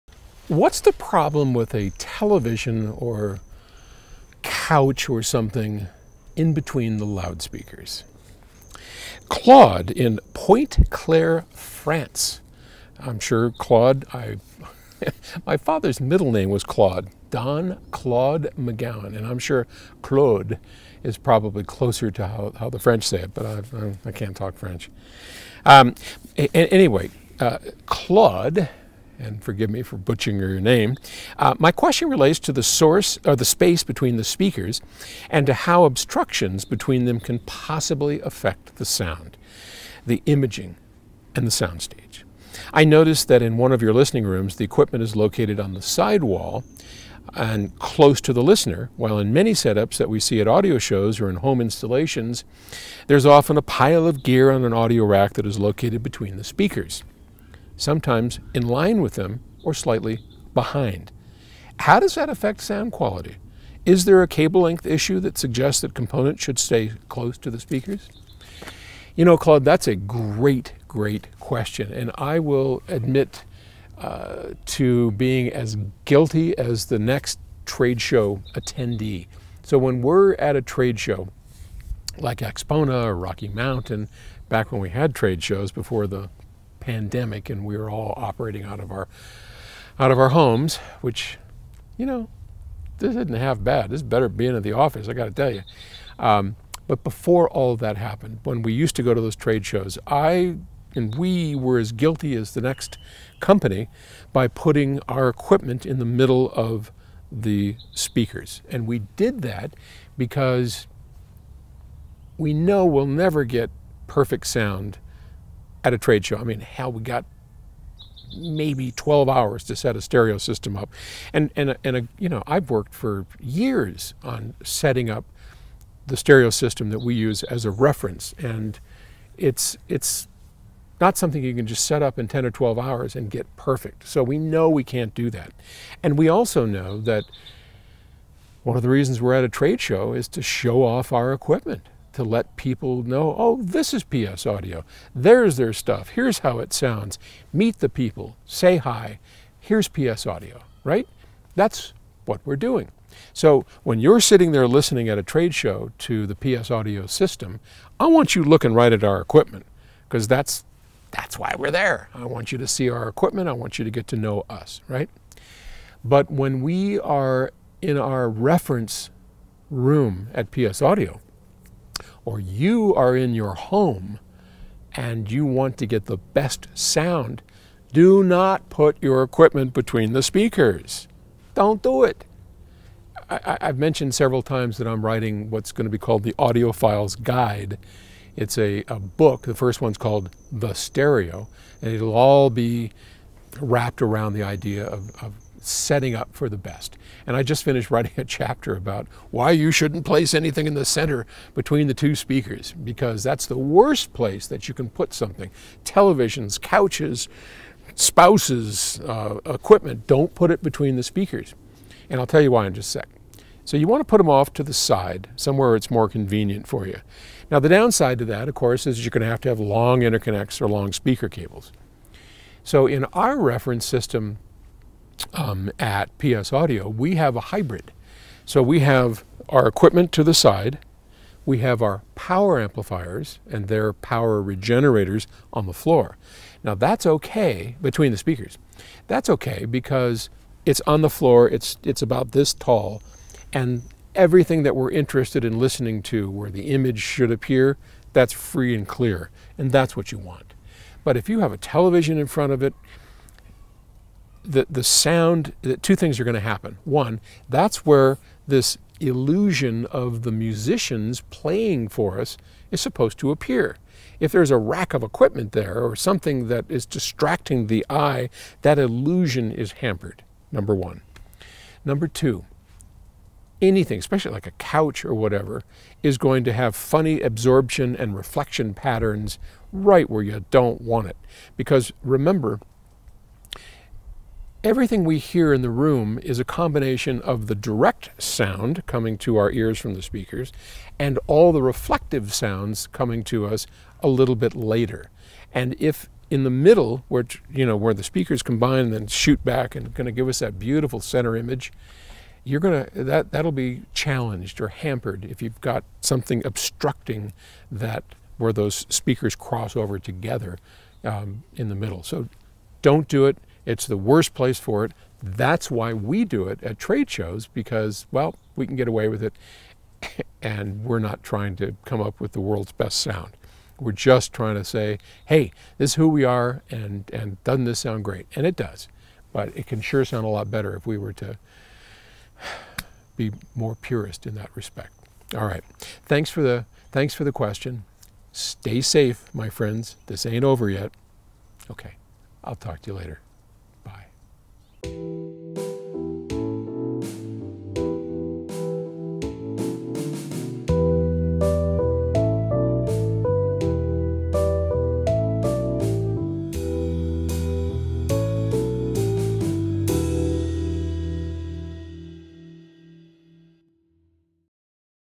The problem with TVs between speakers
If you go to a HiFi tradeshow your likely to see the stereo equipment between the two speakers, but then we hear that's bad. What gives? If you want to learn more, grab a copy of Paul's new book, The Audiophile's Guide.